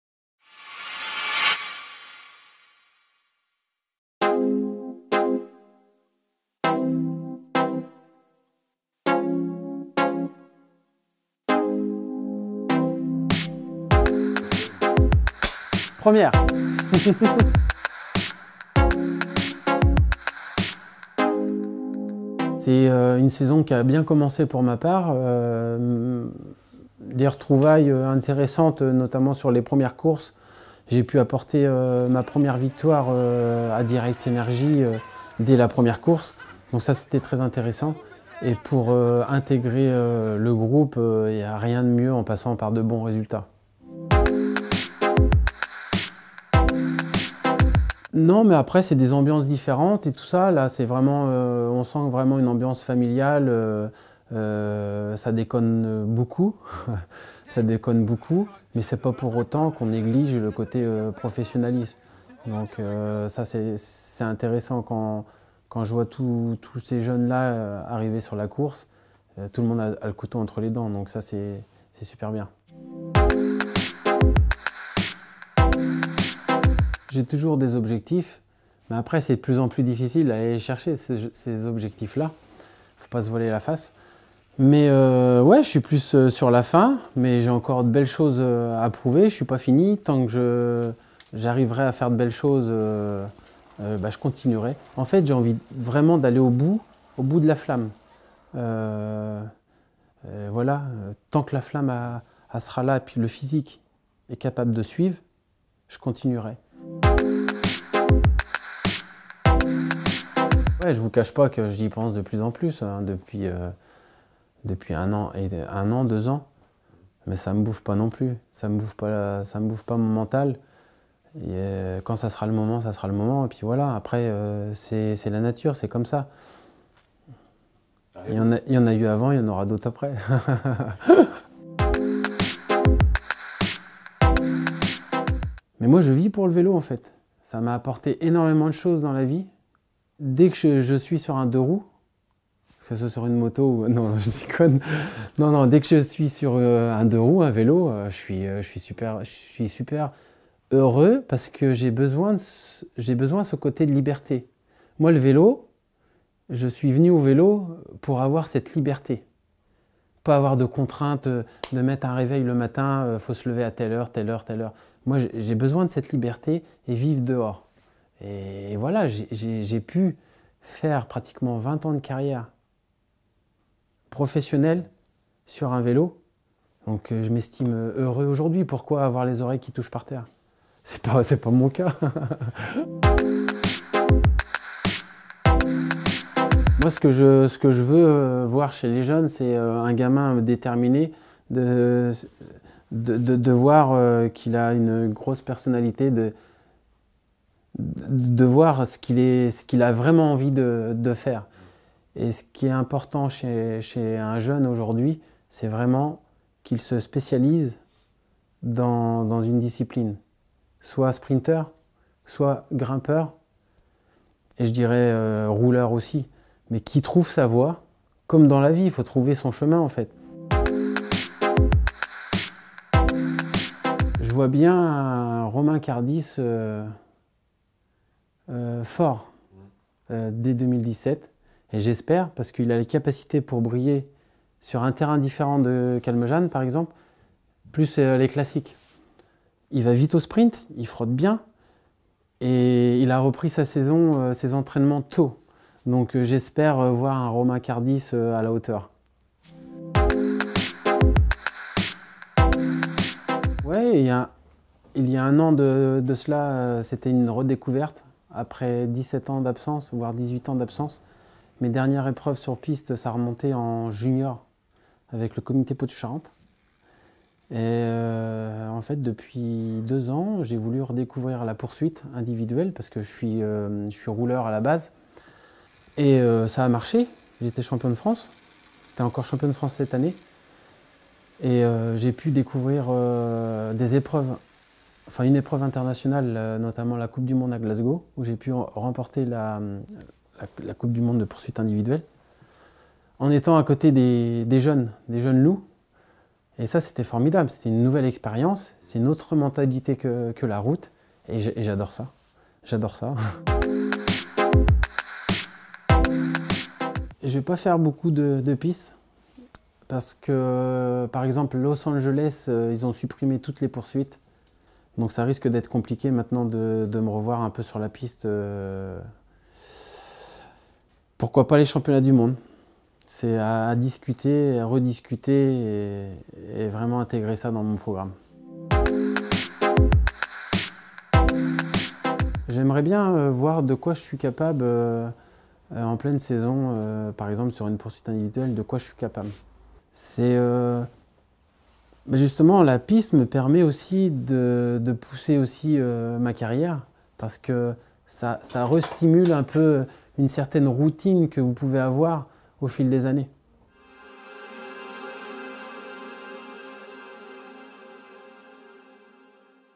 [La Grosse Interview] Sylvain Chavanel
Comment Sylvain Chavanel envisage sa saison 2017 ? Chava a répondu à toutes nos questions !